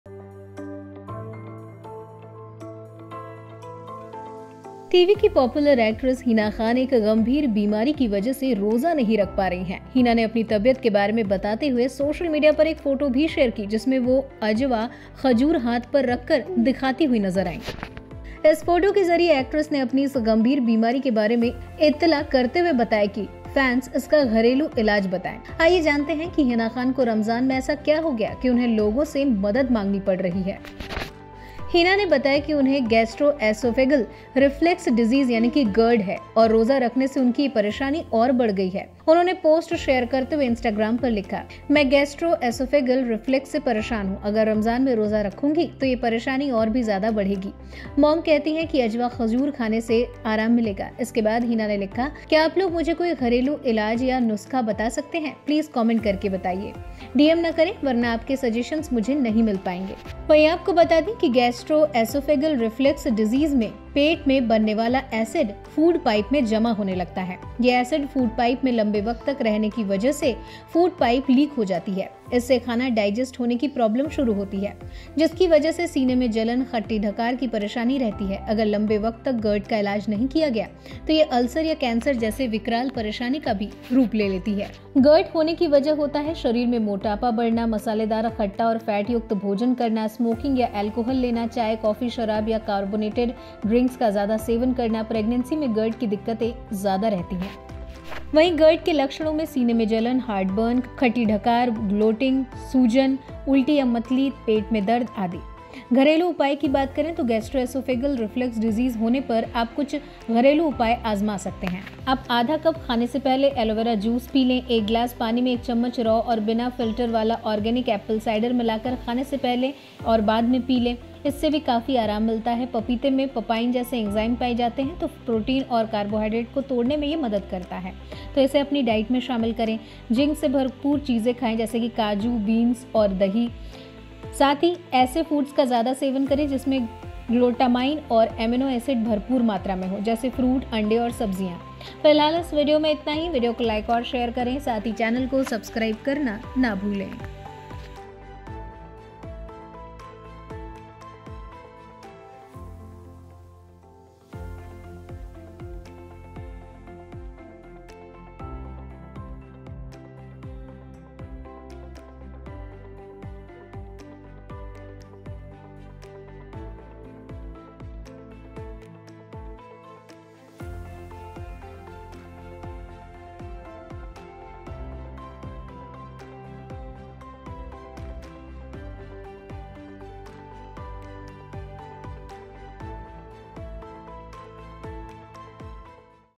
Hina Khan का टूटा Roza, Ramadan 2024 के बीच हुई GERD बीमारी से ग्रस्त Symptoms Reason | Boldsky
Hina Khan Health Update : टीवी की पॉपुलर एक्‍ट्रेस हिना खान एक गंभीर बीमारी के वजह से रोजा नहीं रख पा रही हैं। हिना ने अपनी तबीयत के बारे में बताते हुए सोशल मीडिया पर एक फोटो शेयर की है जिसमें वो अजवा खजूर हाथ पर रखकर दिखाती हुई नजर आईं।इस फोटो के जरिए एक्‍ट्रेस ने अपनी इस गंभीर बीमारी के बारे में इतला देते हुए बताते हुए फैंस से इसका घरेलू इलाज भी पूछा है। आइए जानते हैं क‍ि ह‍िना खान को रमजान में ऐसा क्‍या हो गया क‍ि उन्‍हें लोगों से मदद मांगनी पड़ी।

Hina Khan Health Update: Popular TV actress Hina Khan is not able to keep fast due to a serious illness. Describing her health, Hina has shared a photo on social media in which she was seen holding Ajwa dates on her hand. Through this photo, the actress has given information about her serious illness and has also asked the fans about its home remedy. Let us know what happened to Hina Khan during Ramadan that she had to ask for help from people.